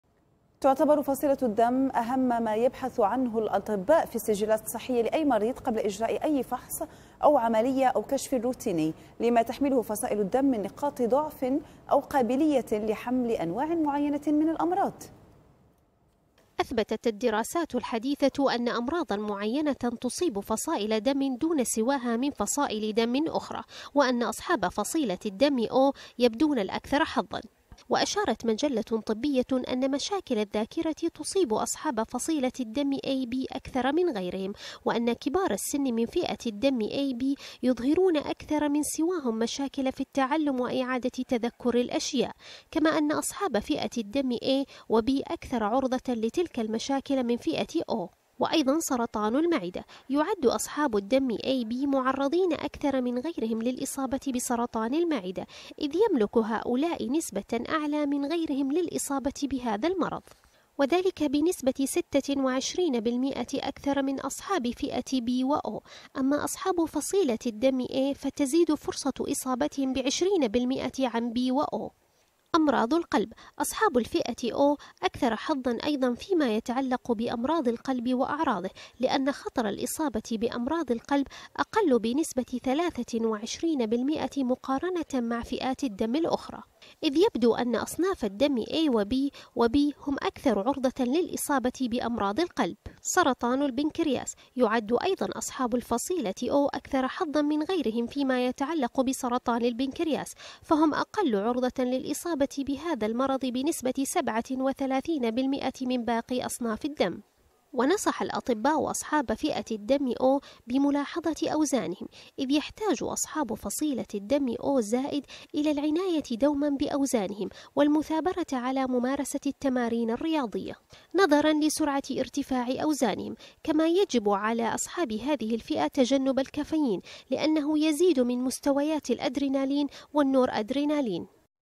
صحتك تهمنا | إذا كانت زمرة دمك O فأنت من أصحاب الفصيلة المحظوظة
تعتبر فصيلة الدم أهم ما يبحث عنه الأطباء في السجلات الصحية لأي مريض قبل إجراء أي فحص أو عملية أو كشف روتيني.
لما تحمله فصائل الدم من نقاط ضعف أو قابلية لحمل أنواع معينة من الأمراض

فقد أثبتت العلماء في دراسة حديثه أن زمرة الدم O  هم الأكثر حظا عن غيرهم من فئات الدم الأخرى..

أثبتت الدراسات الحديثه أن أمراضاً معينة تصيب فصائل دم  دون سواها من فصائل دم أخرى ، و أن أصحاب فصيلة الـدم O يبدون الأكثر حظاً.

وأشارت مجلة طبيه  أن مشاكل الذاكره تصيب أصحاب فصيلة الدم AB أكثر من غيرهم، وأن كبار السن من فئة الدم AB يظهرون أكثر من سواهم مشاكل في التعلم وإعادة تذكر الأشياء.

كما أن أصحاب فئة A وB أكثر عرضة لتلك المشاكل من فئة O.

سرطان المعدة
كذلك يعتبر أصحاب الدم AB معرضين أكثر من غيرهم للإصابة بسرطان المعدة، إذ يملك هؤلاء نسبة أعلى من غيرهم بالإصابة بهذا المرض، وذلك بنسبة 26% أكثر من أصحاب فئة B و O .
أما أصحاب فصيلة الدم A فتزيد فرصة إصابتهم بـ 20% عن B و O .

أمراض القلب
أصحاب الفئة O اكثر حظا أيضا فيما يتعلق بأمراض القلب وأعراضه ، لأن خطر الإصابه بأمراض القلب أقل بنسبة 23% بالمقارنة مع فئات الدم الأخرى،.
إذ يبدو أن أصناف الدم AB و B هم أكثر عرضة للإصابة بأمراض القلب.

سرطان البنكرياس
يعد أيضا  أصحاب الفصيلة O أكثر حظاً من غيرهم في ما يتعلق بسرطان البنكرياس، فهم أقل عرضة للإصابة بهذا المرض بنسبة 37% من باقي أصناف الدم
ونصح الأطباء أصحاب فئة الدم  O  بملاحظة أوزانهم ". إذ يحتاج أصحاب فصيلة الدم o+ إلى العناية دوماً بوزنهم، والمثابرة على ممارسة التمارين الرياضية
نظراً لسرعة ارتفاع وزنهم، كما يجب على أصحاب هذه الفئة تجنب الكافيين لأنه يزيد مستويات الأدرينالين والنورادرينالين.